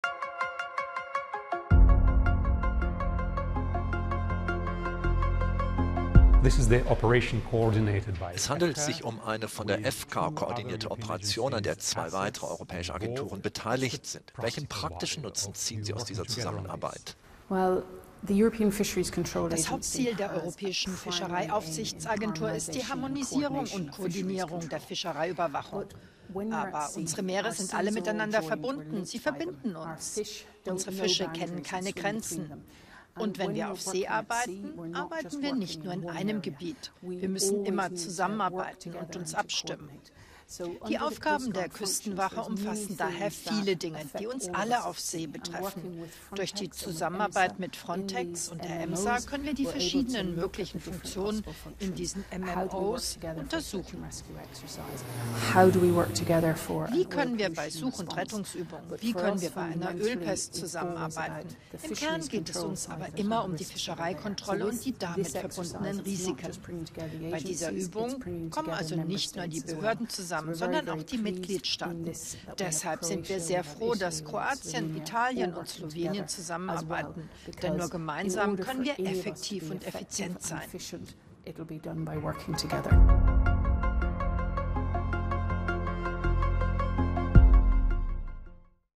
Verbesserte Fischereikontrolle: Die EU setzt auf Zusammenarbeit
Im Bonus-Interview spricht EFCA-Direktorin Dr. Susan Steele mit Euronews über die Vorteile der Mission.

Mit Unterstützung von The European Commission